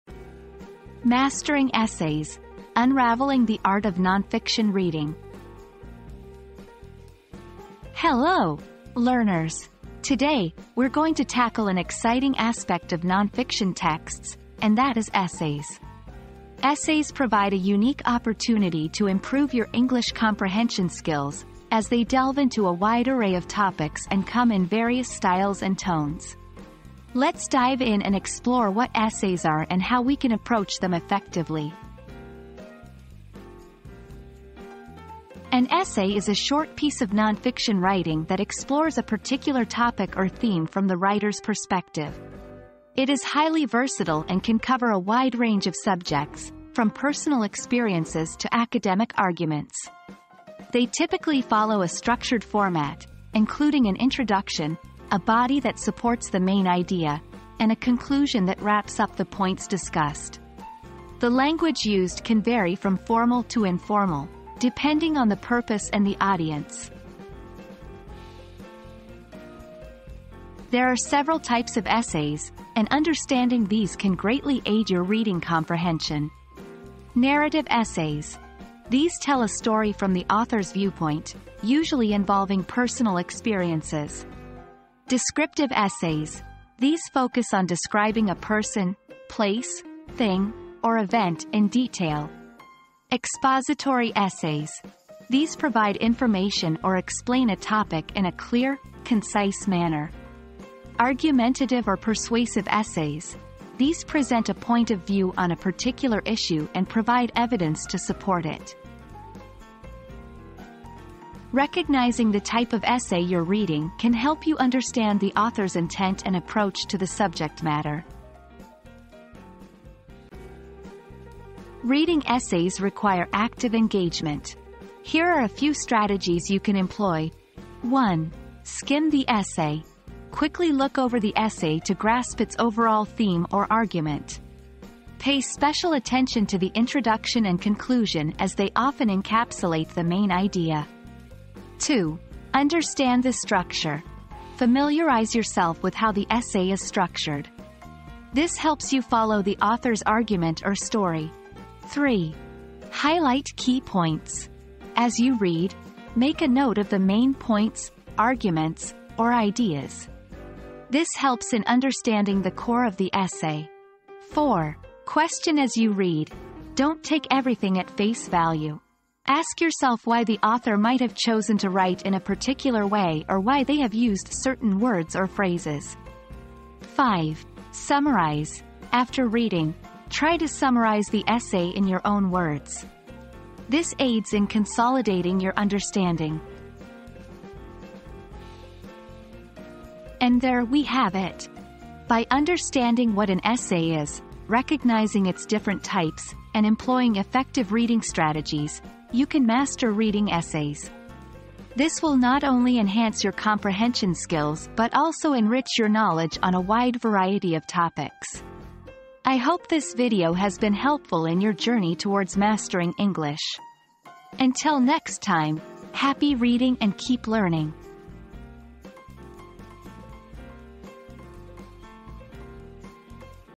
Mastering Essays: Unraveling the Art of Non-fiction Reading
Mastering Essays: Unraveling the Art of Non-fiction Reading • Join us as we delve into the world of essays and discover effective reading strategies to improve your English comprehension skills. Learn about the different types of essays and how to recognize their unique characteristics. Get ready to unravel the art of non-fiction reading!

00:00 • Introduction - Mastering Essays: Unraveling the Art of Non-fiction Reading
00:34 • Understanding What an Essay Is
01:10 • Types of Essays
02:01 • Reading Strategies for Essays